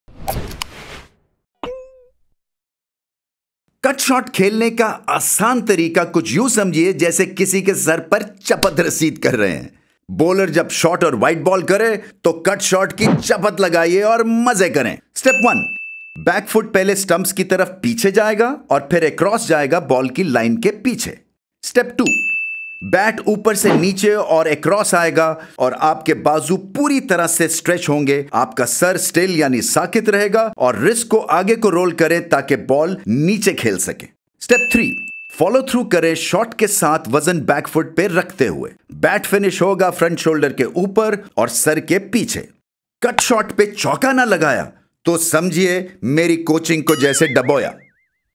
CUT SHOT | Coaching Shoching with Ramiz Raja
Be an expert at playing the Cut Shot, best shot in the book to score a Four..